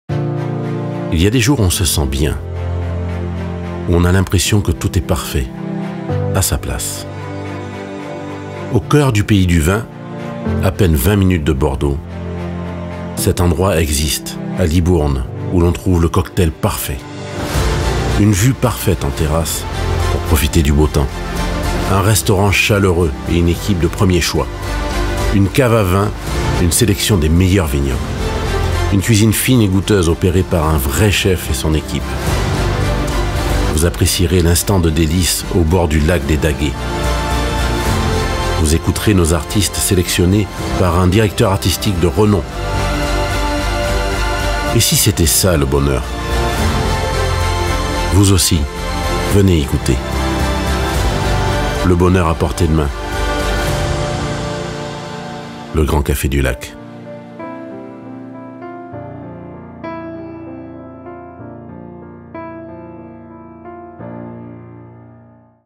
Le Grand café du Lac à Libourne
Le Grand café du Lac à Libourne. Le carrefour des arts, saveurs et musique.
Et si c'était ça le bonheur?...